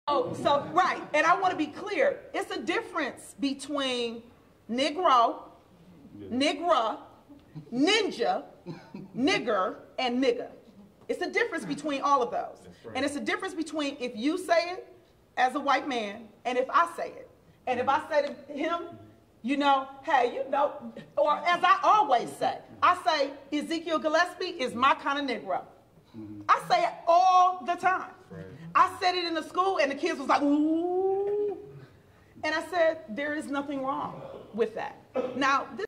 State Senator Lena Taylor comments on use of N-word in black culture
At a news conference Wednesday, State Senator Lena Taylor commented on the use of the N-word within black culture, and the differences in use between white and black speakers. Senator Taylor was responding to a recent bullying allegation as well as incident in which she was accused of calling a bank teller a "house (N-word)."